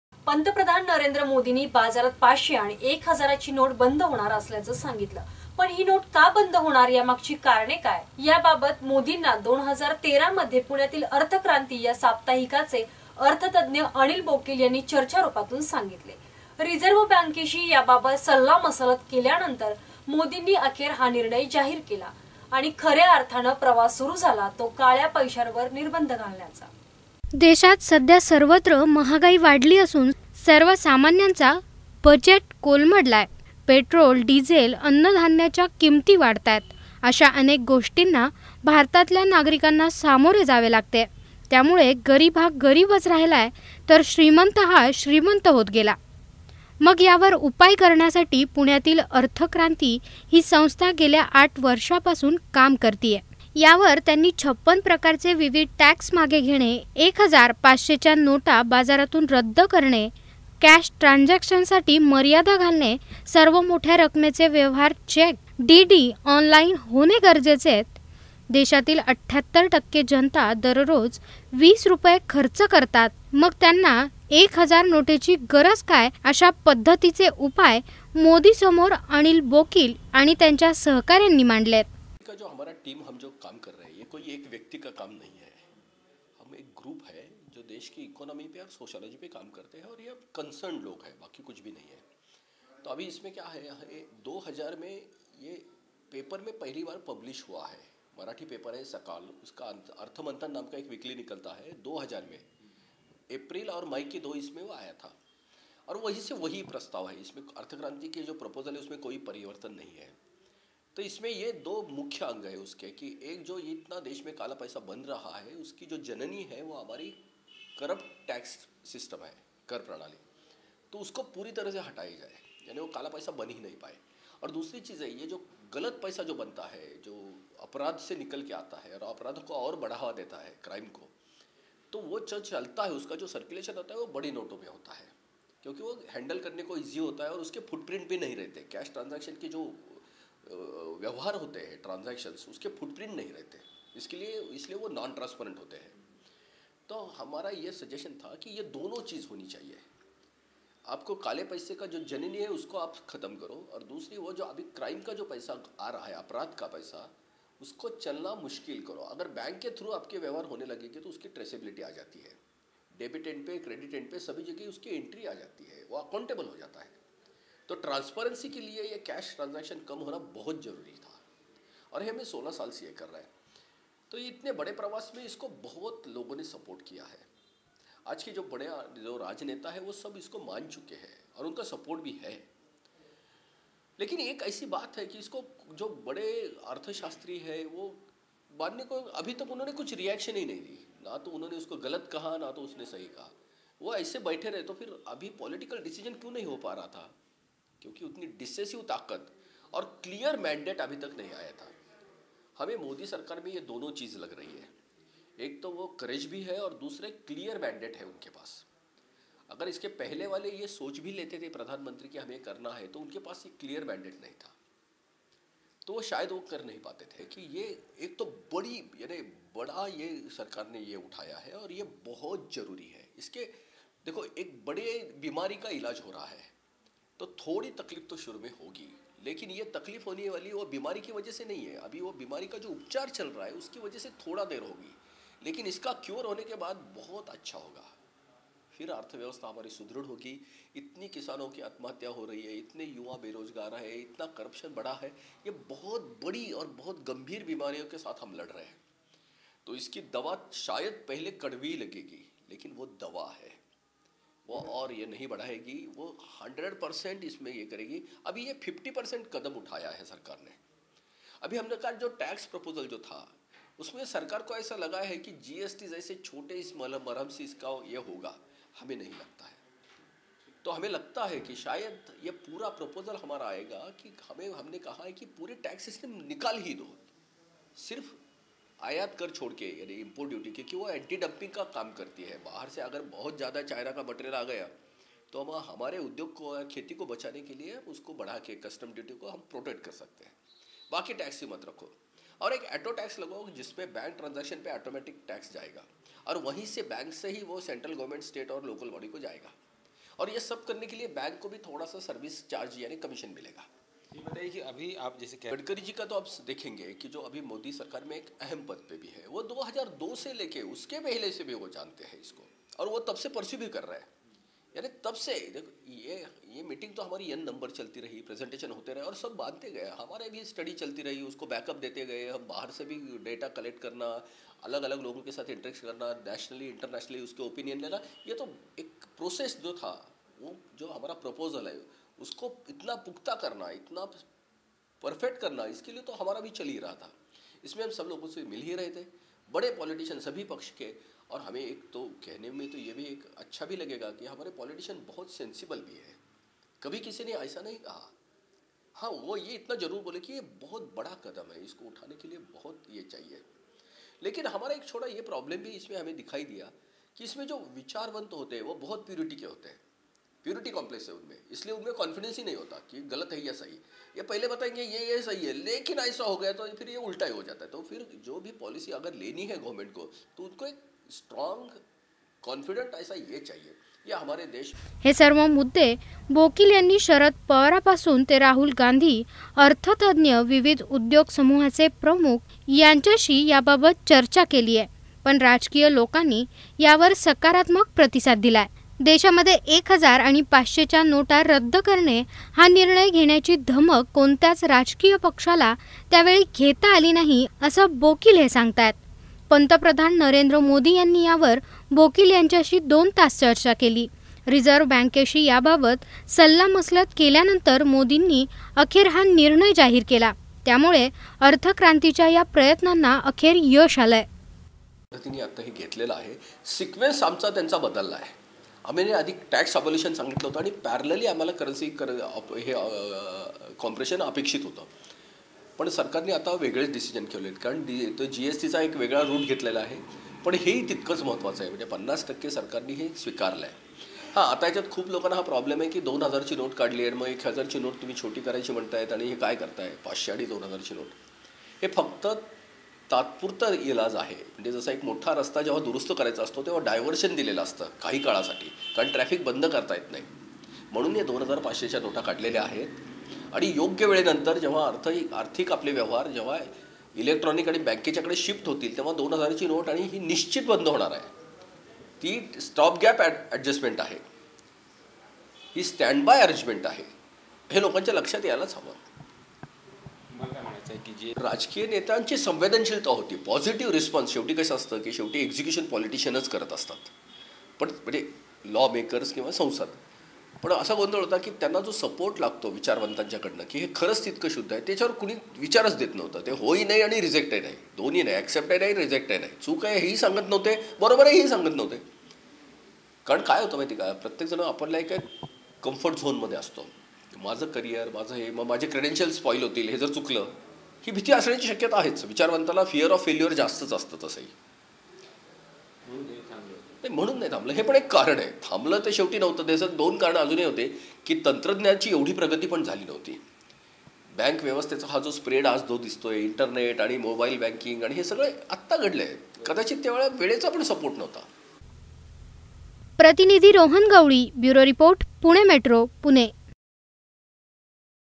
पुण्यातील 'अर्थक्रांती' या संस्थेचे अर्थतज्ञ अनिल बोकील यांनी घेतली नरेंद्र मोदी यांची भेट
पंतप्रधान नरेंद्र मोदीनी बाजारात पाचशे आणि एक हजार ची नोट बंद होणार असल्याचे सांगितले. पण ही नोट का बंद होणार यामागची कारणे काय  याबाबत मोदींना २०१३ मध्ये पुण्यातील अर्थक्रांतीचे या साप्ताहिकाचे अर्थतज्ञ अनिल बोकील यांनी चर्चा रूपातून सांगितले रिझर्व बँकेशी याबाबत सल्लामसलत केल्यानतंर मोदी अखेर हा निर्णय जाहीर केला आणिख-या अर्थाने प्रवास सुरु झाला तो काळ्या पैशावर निर्बंध घालण्याचा.